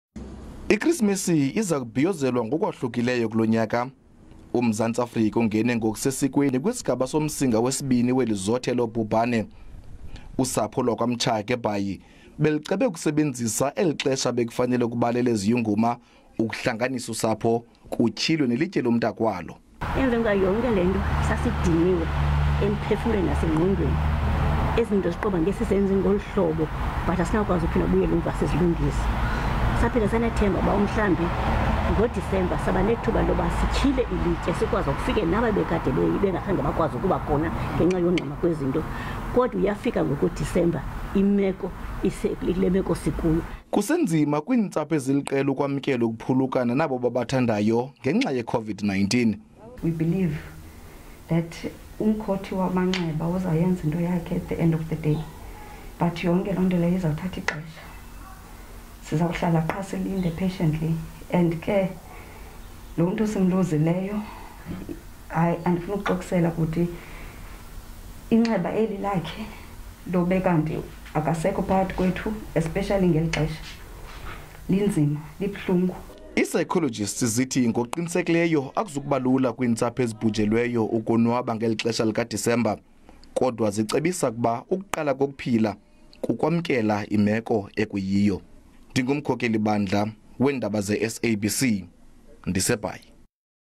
Luzoba muncu usuku lukaKhisimusi kulonyaka
Luzoba muncu usuku lukaKhisimusi kulonyaka njengoba ludla lubi ubhubhane lwesifo iCOVID-19. Ikakhulu ngoba kweminye imindeni ludle namanye amalungu.